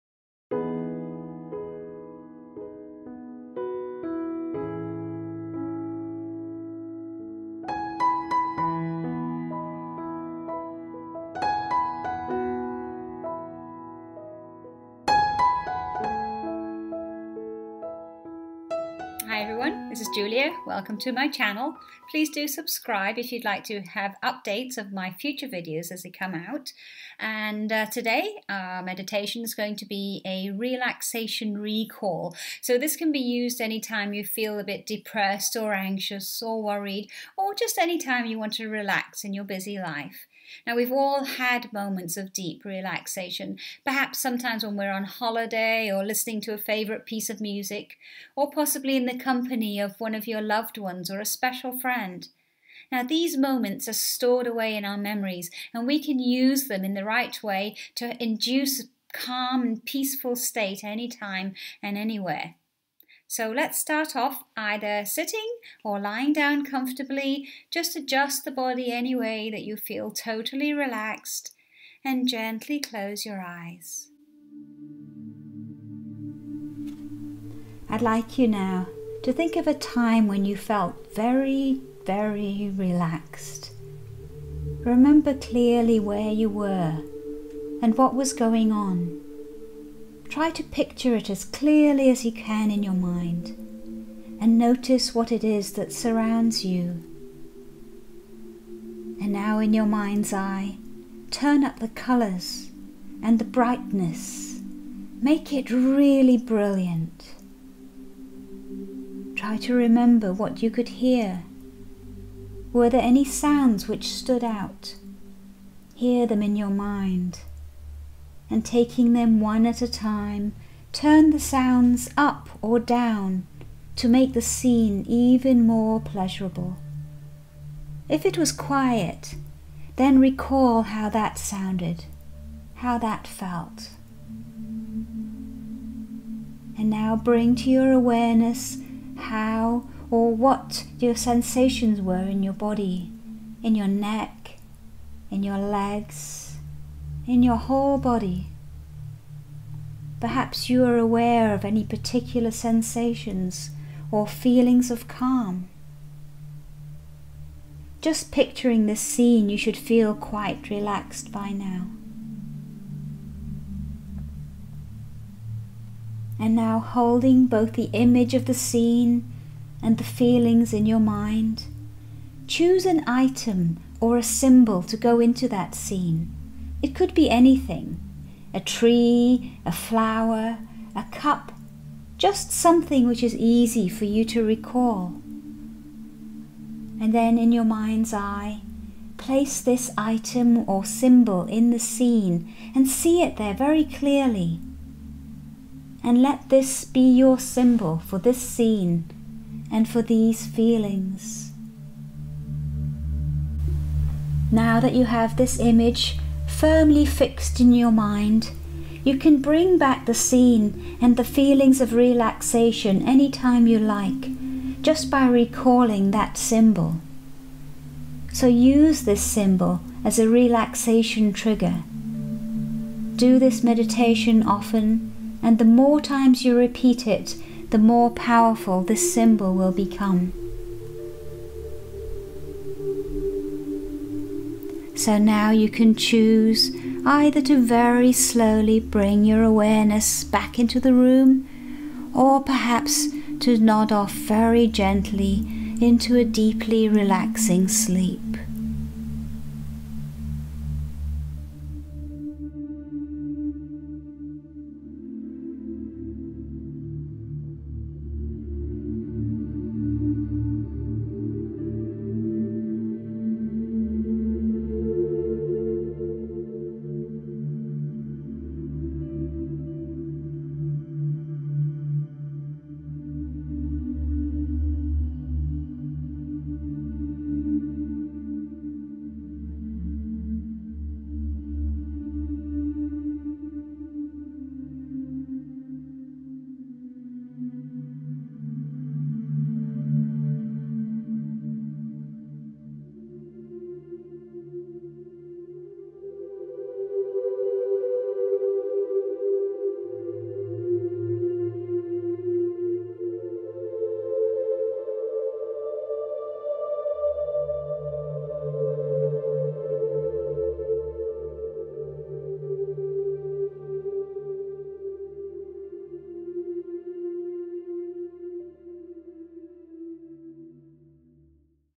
5 minute Guided Relaxing Meditation | Stress and Anxiety help
This is a 5 minute guided relaxing meditation for relaxing and to help lower stress and anxiety levels.  Listen to the meditation music and sounds of my voice to guide you gently into relaxation.
In this short meditation you will learn how to trigger the relaxation response any time you want.

Please enjoy my free yoga and meditation channel.

This is part of the free meditation series: 5 Minute Meditations with Julia Wylie